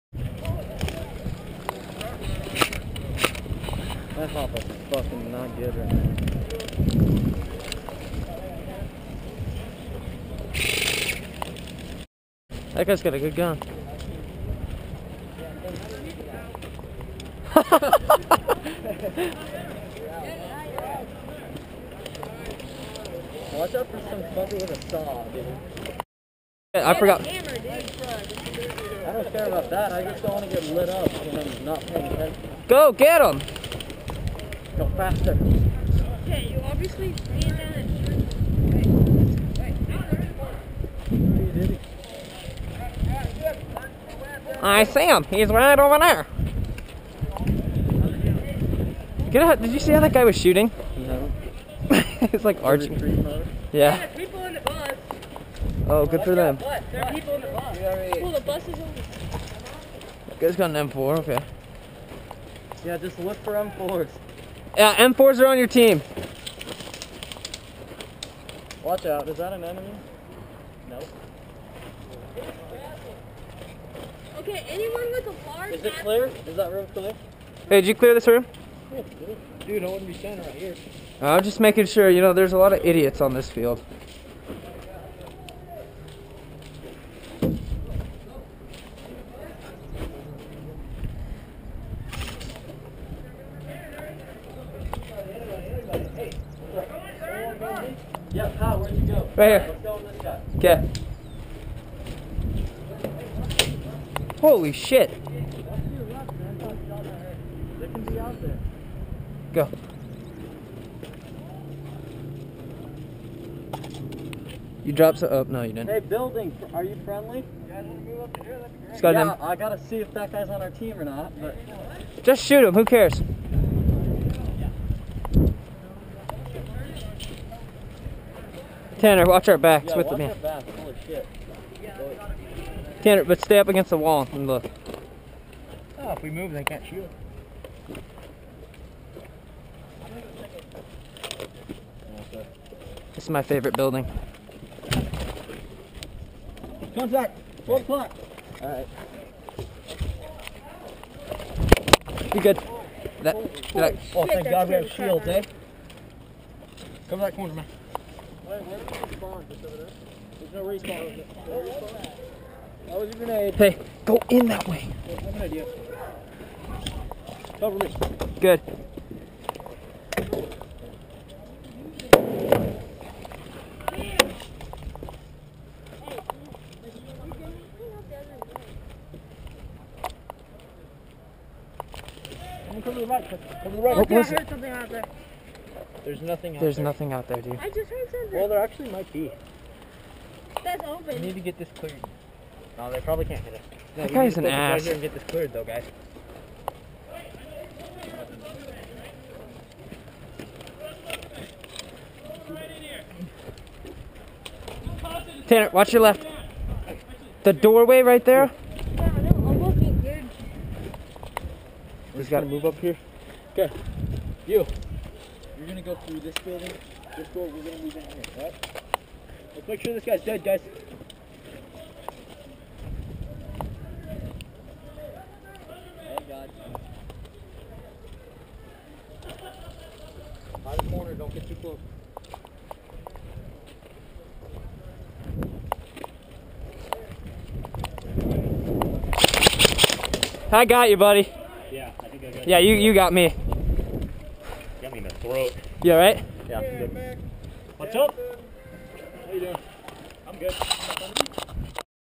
Jericho Lake Elsinore 6-25-11 (Specialists)
This was the last game of the day. There were no respawns. It was the specialists (all those using SAWs, snipers, pistols, shotguns, and knives) against everyone else.